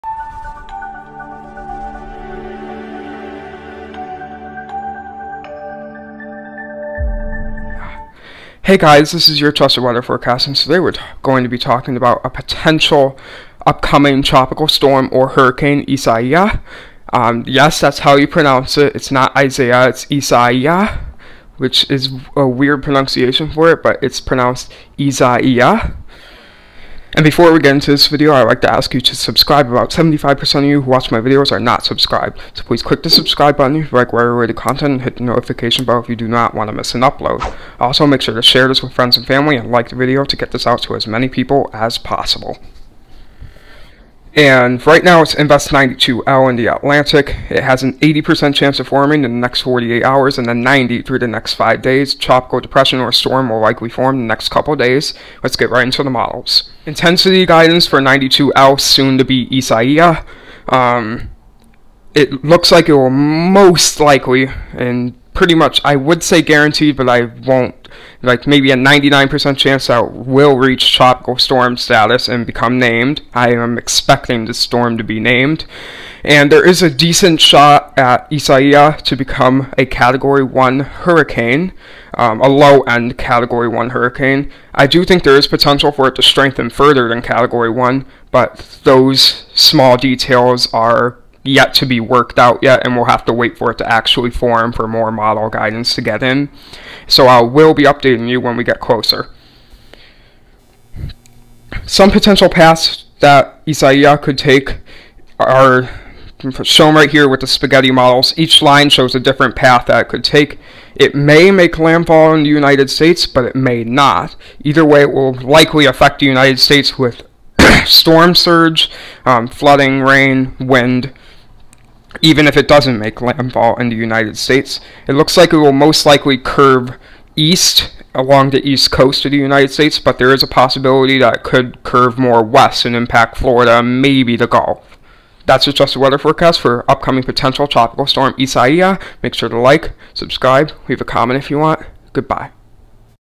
Potential Tropical Storm Isaias Forecast (Hurricane Possible?)
Likely tropical storm Isaias will form within the next couple of days before potentially strengthening into a hurricane, there is slight potential for this system to become a major hurricane,  but those details have yet to be worked out. Isaias will likely impact the United States in early August with storm surge, flash flooding and gusty winds. There is plenty of uncertainty with this system and updates will be provided later. There is a chance that it doesn’t even form. But will still need to be watched closely.